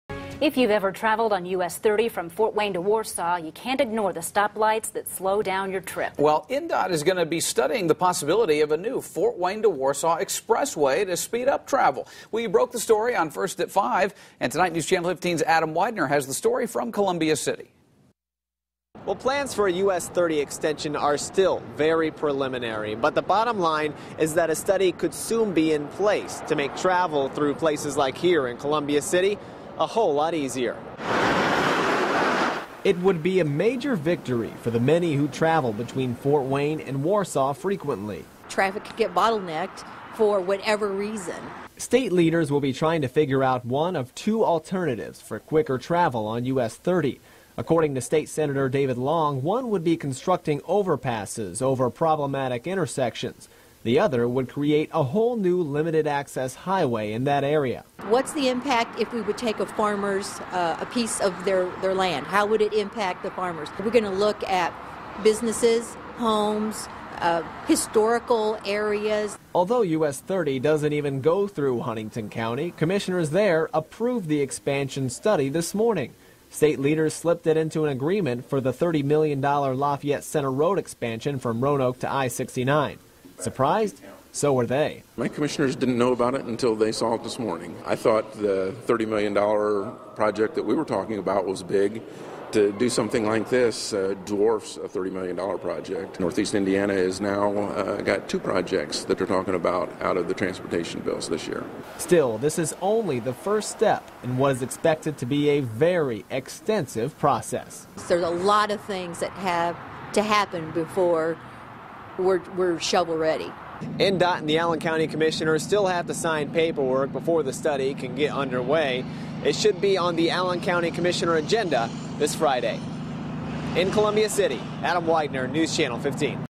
INDOT to study new U.S. 30 expressway
State officials are gearing up to study an extension to U.S. 30 between Fort Wayne and Kosciusko County that would bypass stoplight-ridden areas like Columbia City and Warsaw.